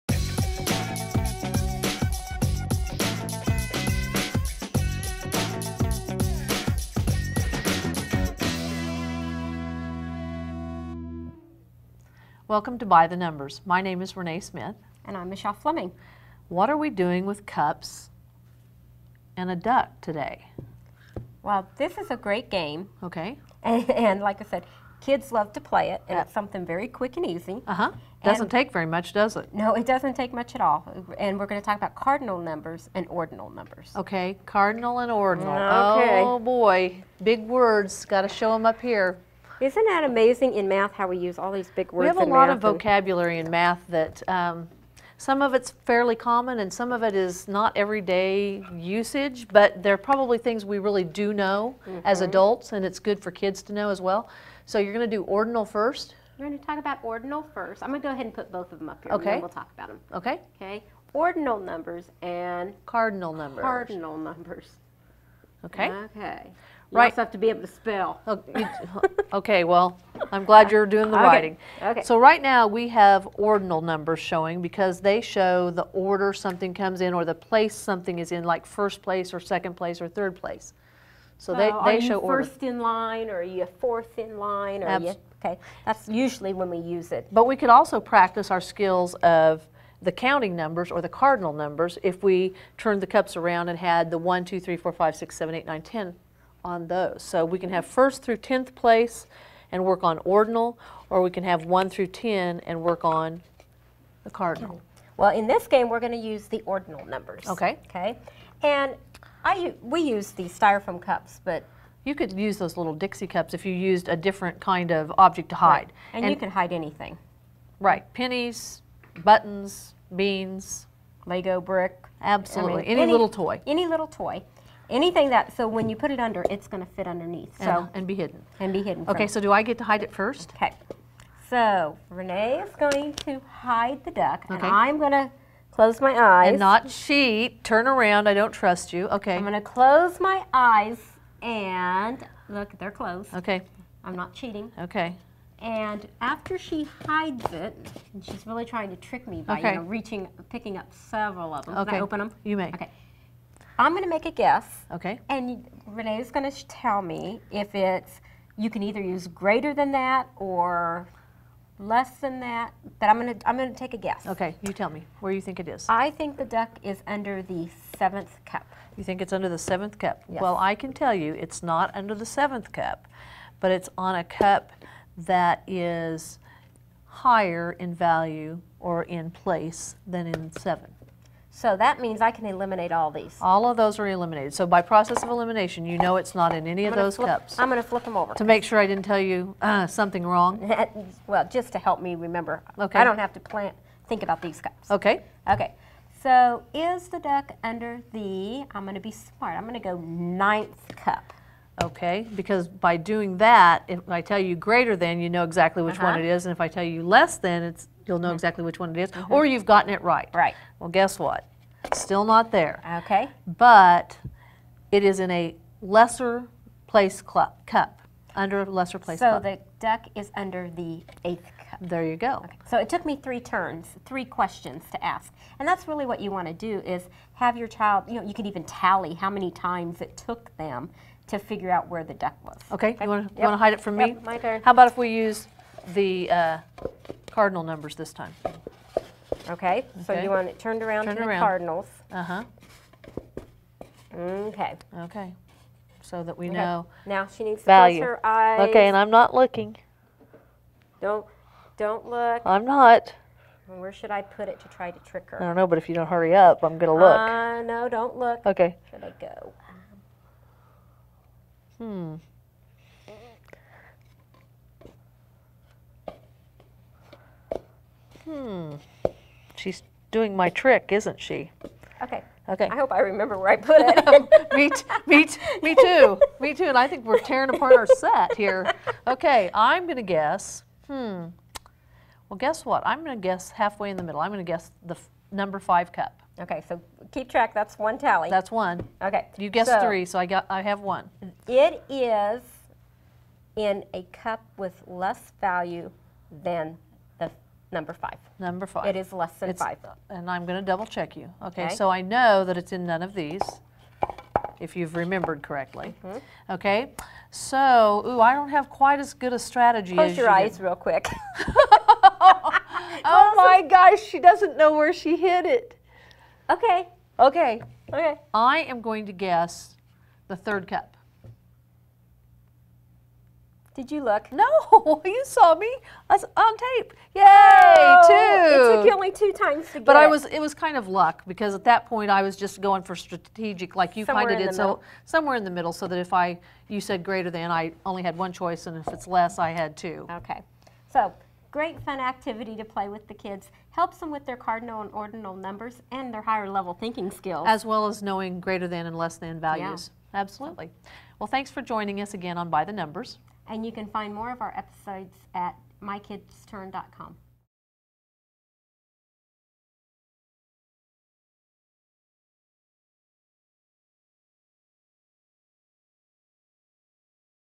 By The Numbers - Cardinal and Ordinal Numbers
Michelle and Renee play a game that works great to help teach kids about cardinal and ordinal numbers.